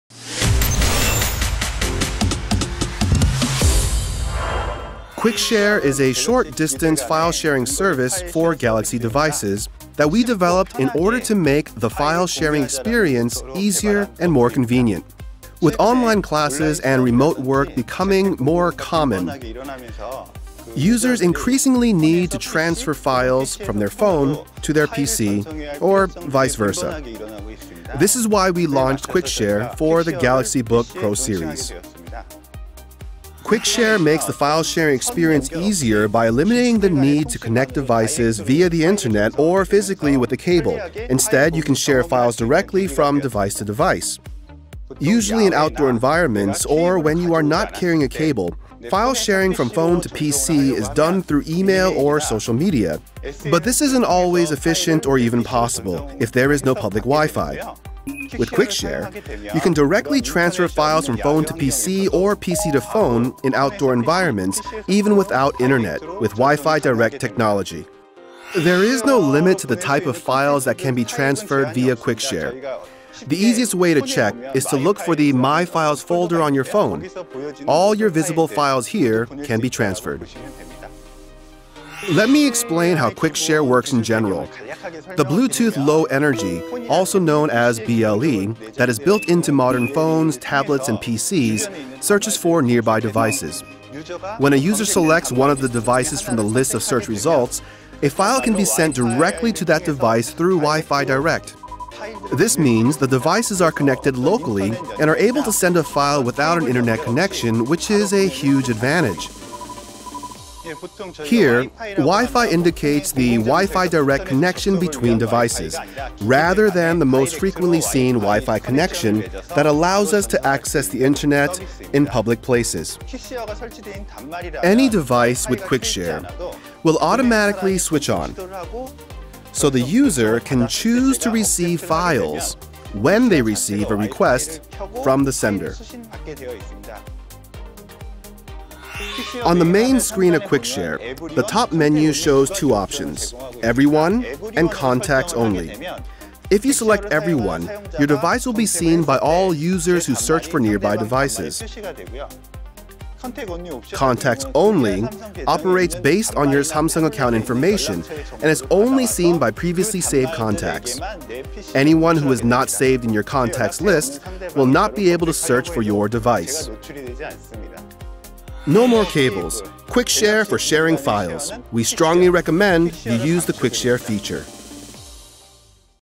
[Galaxy Book Pro Series Tech Talk] ③ Quick Share
Q&A: Samsung Electronics developers on the strengths of the Galaxy Book Pro series.

Q. What is Quick Share technology?
Quick Share is a short-distance file sharing service for Galaxy devices that we developed in order to make the file sharing experience easier and more convenient.

With online classes and remote work becoming more common, users increasingly need to transfer files from their phone to their PC, or vice versa. This is why we launched Quick Share for the Galaxy Book Pro series.

Q. When and where would Quick Share be especially useful?
Quick Share makes the file sharing experience easier by eliminating the need to connect devices via the internet or physically with a cable. Instead, you can share files directly from device to device.

Usually in outdoor environments or when you are not carrying a cable, file sharing from Phone to PC is done through email or social media. But this isn’t always efficient or even possible if there is no public Wi-Fi. With Quick Share, you can directly transfer files from Phone to PC, or PC to Phone in outdoor environments even without internet with Wi-Fi Direct technology.

Q. What kind of files can you transfer with Quick Share?
There is no limit to the type of files that can be transferred via Quick Share. The easiest way to check is to look for the ‘My Files’ folder on your phone. All your visible files here can be transferred.

Q. What is the technology that makes Quick Share possible?
Let me explain how Quick Share works in general. The Bluetooth Low Energy, also known as BLE, that is built into modern phones, tablets and PCs searches for nearby devices. When a user selects one of the devices from the list of search results, a file can be sent directly to that device through Wi-Fi Direct. This means the devices are connected locally and are able to send a file without an internet connection, which is a huge advantage.

Here, Wi-Fi indicates the Wi-Fi Direct connection between devices, rather than the more frequently seen Wi-Fi connection that allows us to access the internet in public places. Any device with Quick Share will automatically switch on, so the user can choose to receive files when they receive a request from the sender.

Q. Can I block my phone from getting files from strangers?
On the main screen of Quick Share, the top menu shows two options: ‘Everyone’ and ‘Contacts only.’ If you select ‘Everyone’, your device will be seen by all users who search for nearby devices. ‘Contacts only’ operates based on your Samsung account information and is only seen by previously saved contacts. Anyone who is not saved in your contacts list will not be able to search for your device.

Q. Is there anything else users should know about Quick Share?
No more cables! Use Quick Share for sharing files between Galaxy devices!
We strongly recommend you use the Quick Share feature.